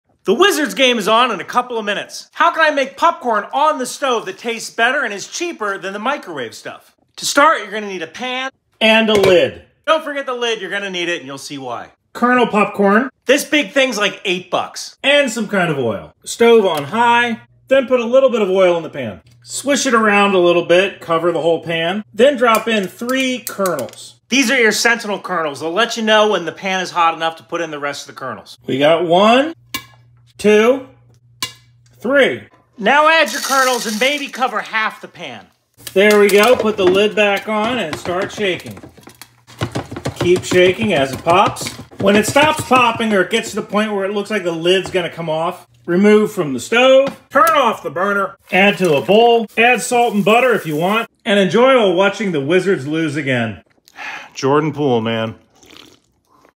How to make pan popcorn
For this you need:
Pan with a lid
Kernel popcorn
Oil
Salt / Butter to taste

Turn the burner on high, ad about a tablespoon of oil and coat the bottom of the pan.
Add three kernels and put the lid on.
When the three kernels pop, cover about half the pan in kernels. Replace the lid.
Pop until the colonel stop popping or the lid starts to lift off the pan.
Add to a container and add salt and butter to taste.